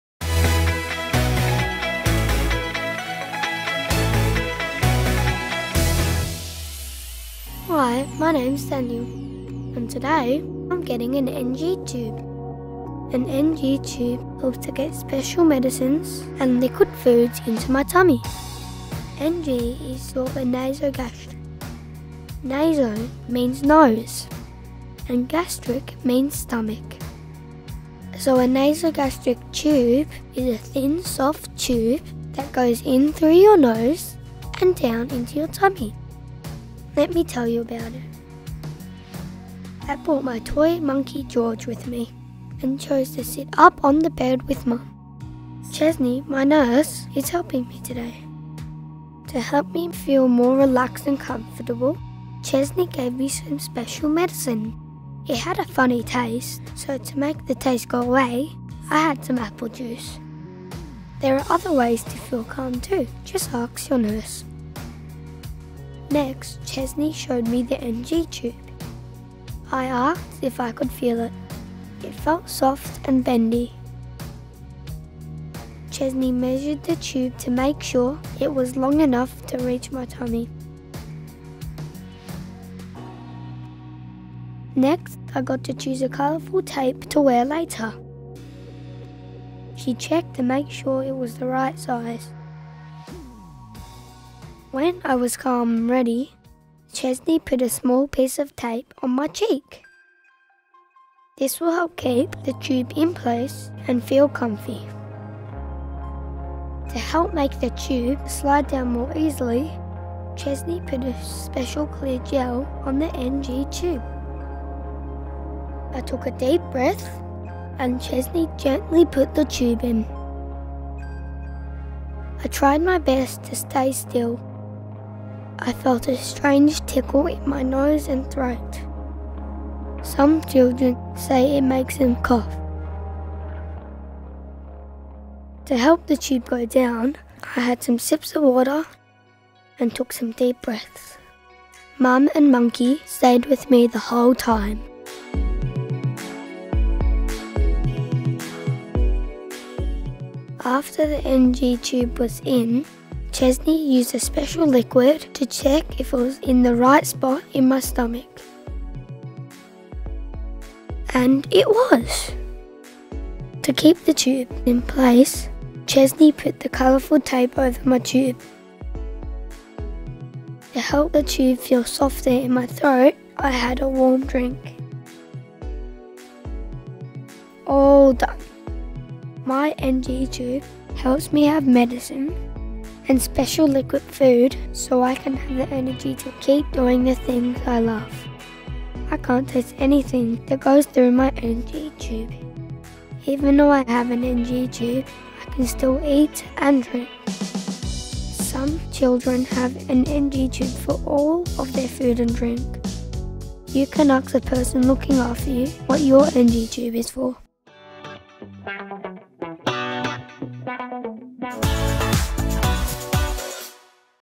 A child's guide to hospital: NG (nasogastric) Tubes
Daniel shows us what it's like to get a nasogastric, or NG, tube.

If you have any feedback or questions about this video please send an email to: be [dot] positive [at] rch [dot] org [dot] au